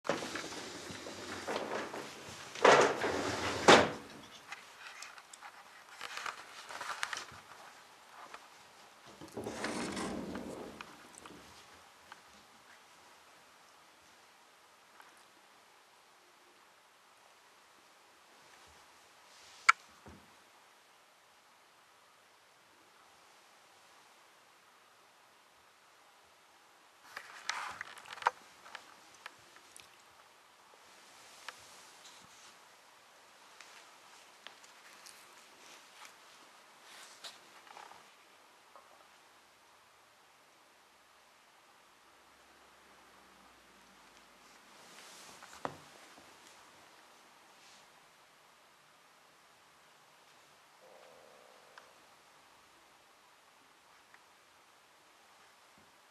帽子を渡さないミーミー
連休は農作業で外に出ることが多いのでいつも帽子は手放せません。昼飯が終わり田んぼに行こうとすると甘えて出てきたミーミーに帽子を占拠されてしまいました。